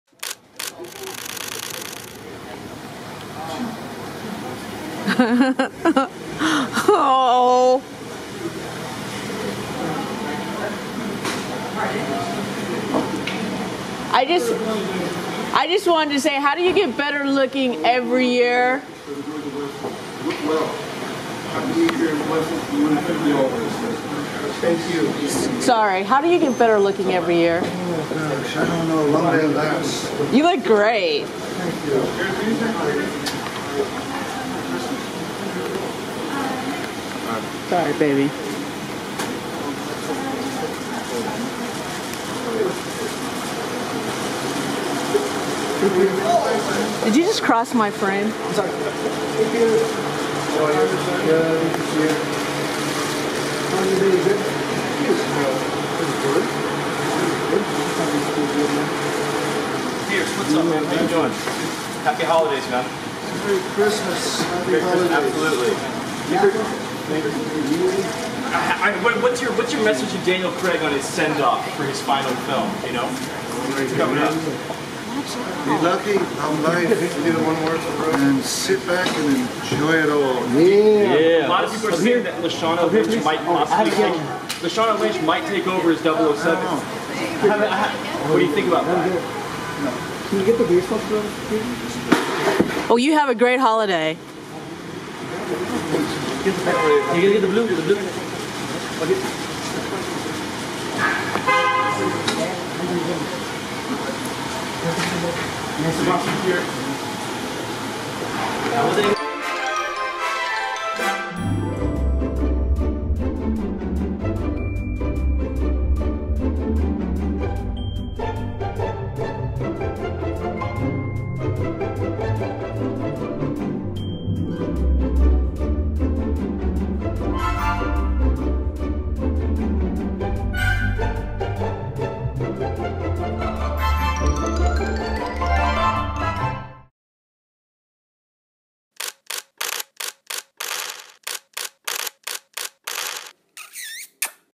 Pierce Brosnan Asked About Daniel Craig's Final Bond At Valet
Pierce Brosnan was asked about Daniel Craig's final Bond at a valet after dining with friend in Los Angeles.  This video is from December 2019, hence the lack of masks!

We must agree with the videographer.  Pierce Brosnan does seem to get more and more handsome.  He also signed autographs for fans.

Who. Brosnan

What. Trying to get his car

Wearing. A suit

Where? 362 N Canon Dr, Beverly Hills, California, USA

You can also visit our Celebrity WotNot website for more great celebrity fun: